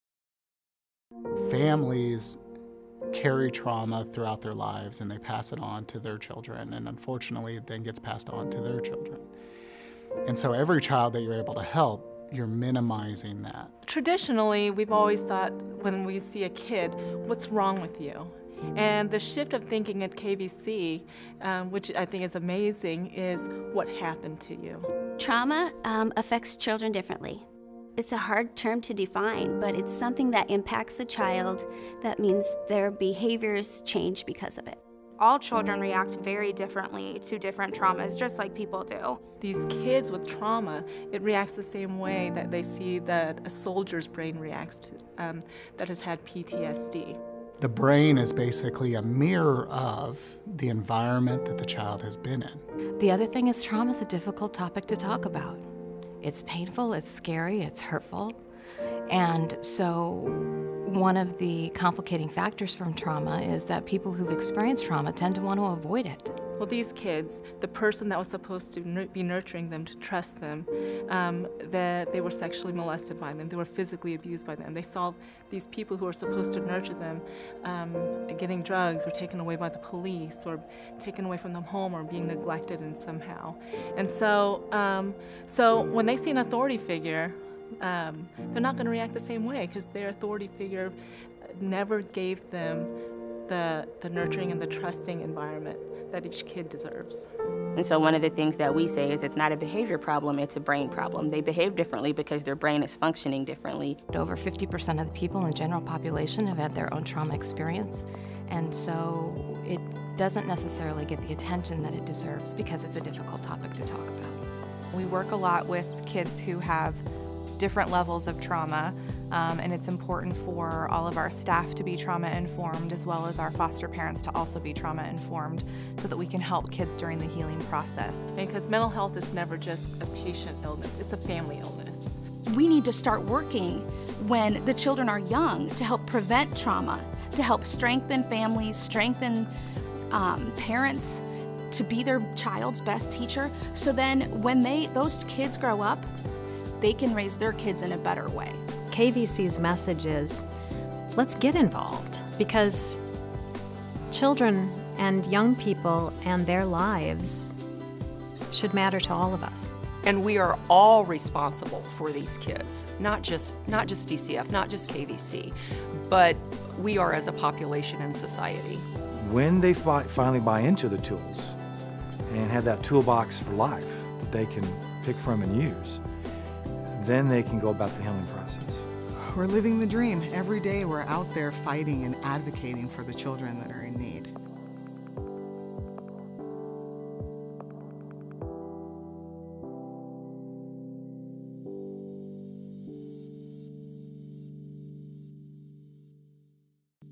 Healing from the Life-Long Effects of Childhood Trauma
Years of research show that some of the worst health and social problems arise from traumatic childhood experiences, and it is estimated that 2 out of 3 youth will be exposed to trauma before the age of 16. Traumatic childhood experiences have life-long effects and impact important regions of the brain responsible for problem solving, emotion regulation and memory. Armed with this information, KVC has integrated trauma-informed care into its child welfare and behavioral health systems to better help children and families understand and heal from trauma.